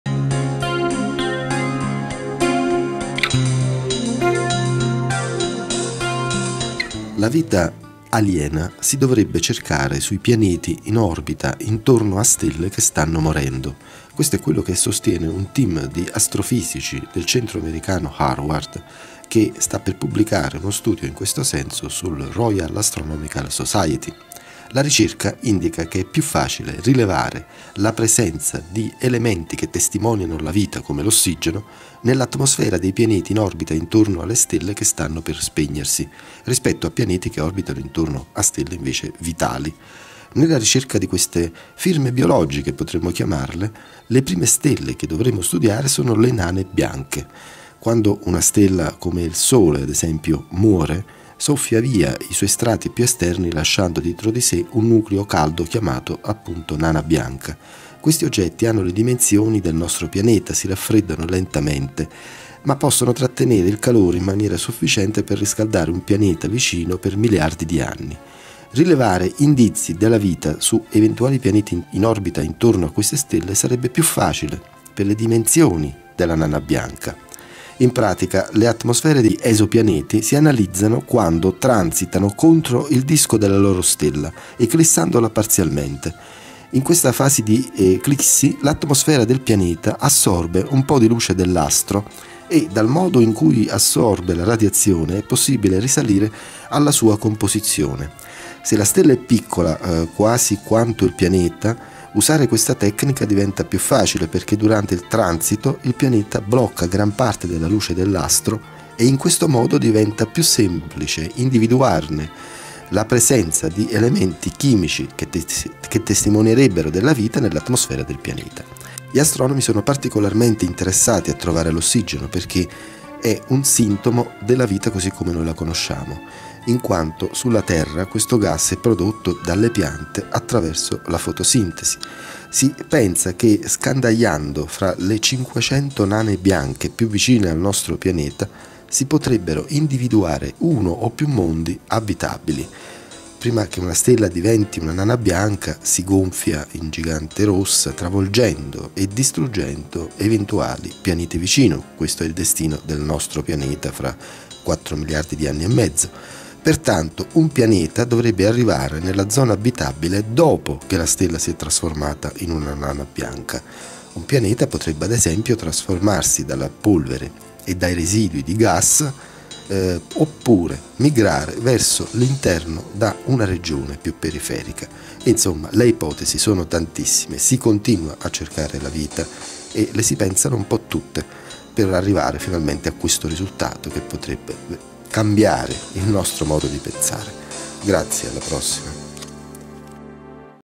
VITA ALIENA ATTORNO A STELLE MORENTI
La vita aliena si dovrebbe cercare sui pianeti in orbita intorno alle stelle morenti. E' quanto sostiene uno studio condotto dagli astrofisici del centro di ricerca americano Harvard-Smithsonian. In via di pubblicazione sulla rivista Monthly Notices of the Royal Astronomical Society, la ricerca indica che è più facile rilevare le firme della vita, come l'ossigeno, nell'atmosfera dei pianeti in orbita intorno alle stelle che stanno ormai per spegnersi, rispetto ai pianeti in orbita attorno alle stella simili al Sole. ''Nella ricerca di firme biologiche extraterrestri, le prime stelle che dovremmo studiare sono le nane bianche'', ha osservato uno degli autori, Avi Loeb.